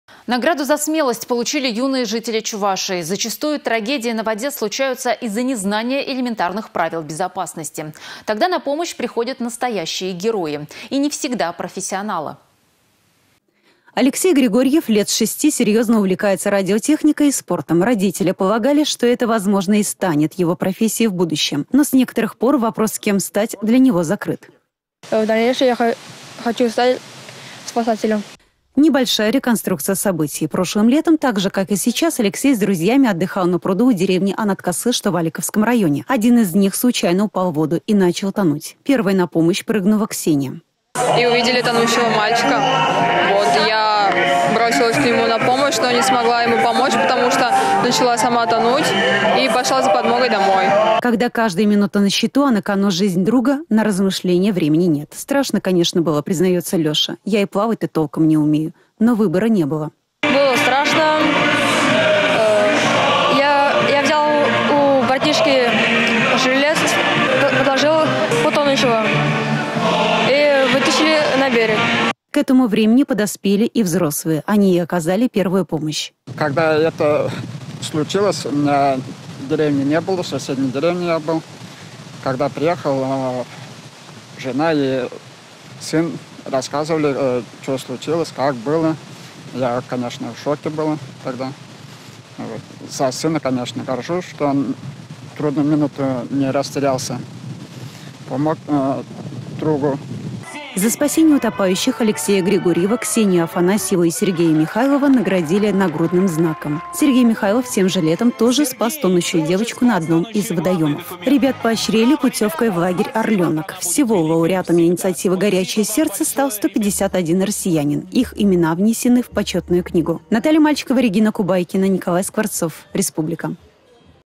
Три школьника республики стали обладателями нагрудного знака «Горячее сердце»
Награду за смелость получили юные жители Чувашии.  Зачастую трагедии на воде случаются из-за незнания элементарных правил безопасности. Тогда на помощь приходят настоящие герои - и не всегда профессионалы.